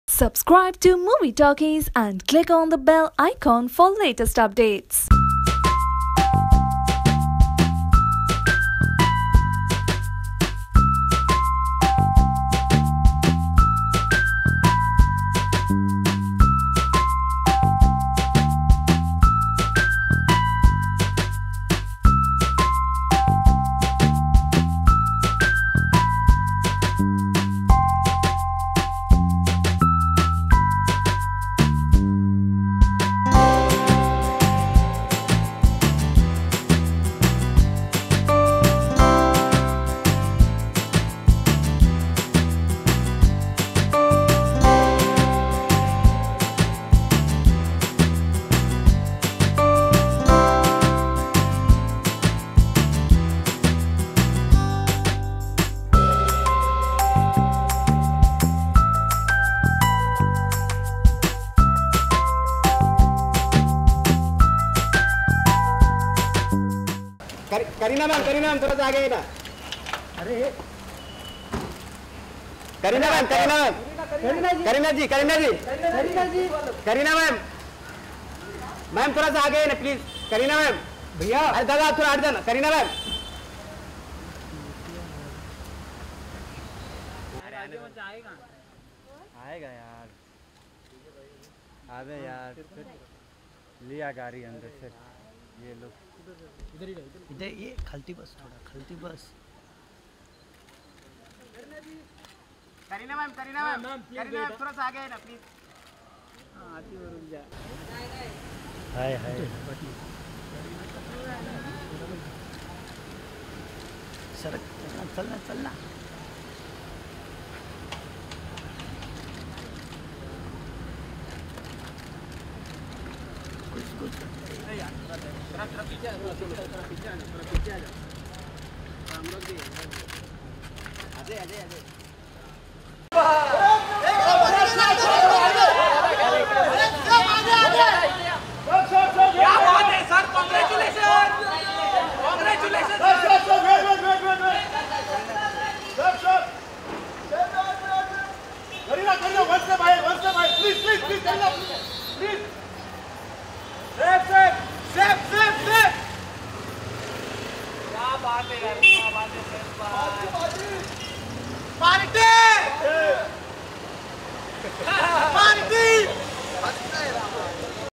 Saif & Kareena Kapoor Take Baby Taimur On His First International Holiday
Saif Ali Khan & Kareena Kapoor were spotted with their cute baby Taimur Ali Khan at Mumbai airport.